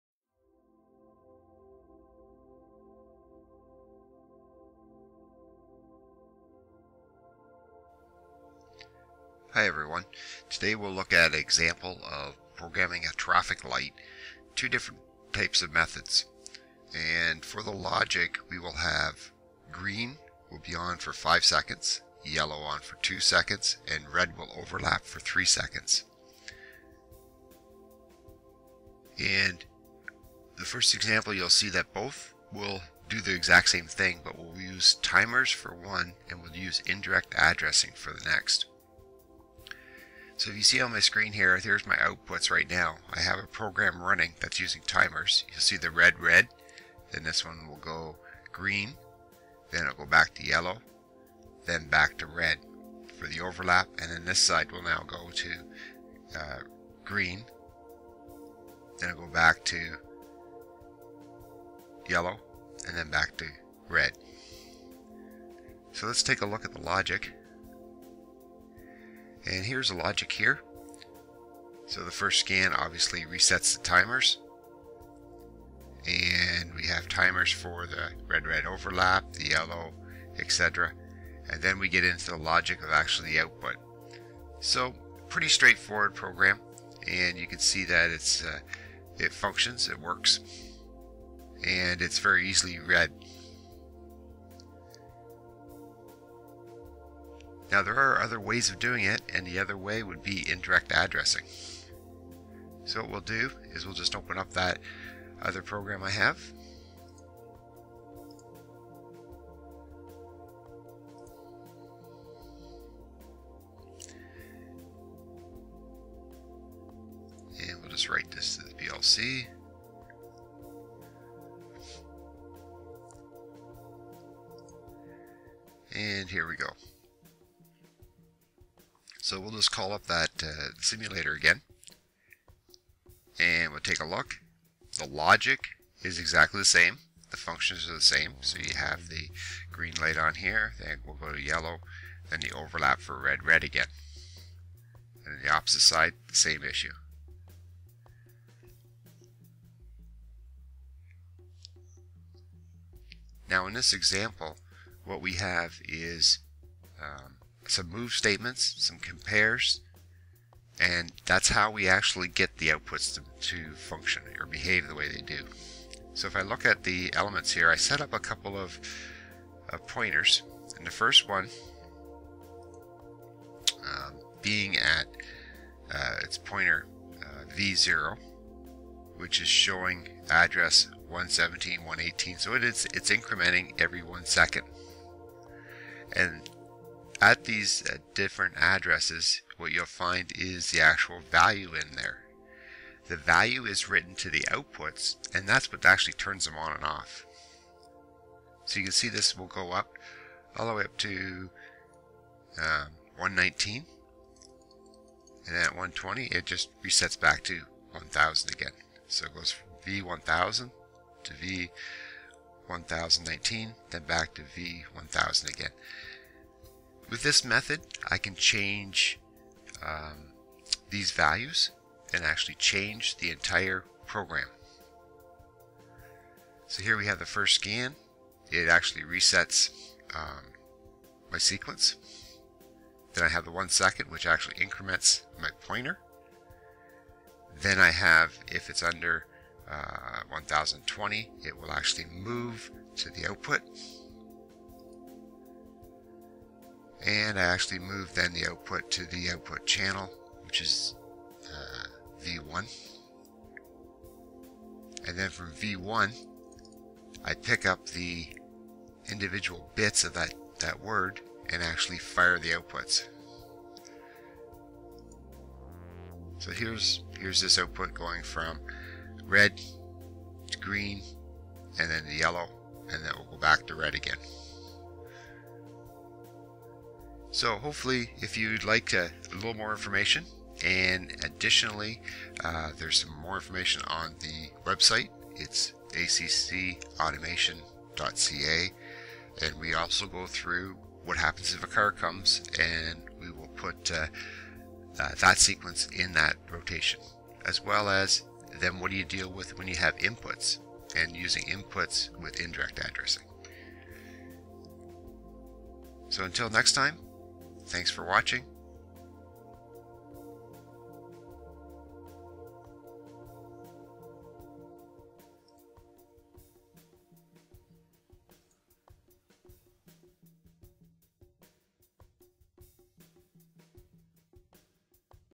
Building a PLC Program that you can be Proud of - Traffic Light
PLC Programming of Traffic Lights
We will look at three programming examples for the lights. Two different approaches to programming will be used, but the program function is the same.
The last example will modify the logic for a car being sensed.

00:00 Building a PLC Program that you can be Proud of - Traffic Light
00:28 Traffic Light Program Ladder Logic Basic (Bits / Timers)
01:38 Traffic Light Program Indirect Addressing

This Building a PLC Program Series can be found at the following locations:

3D simulation software showing the traffic light program.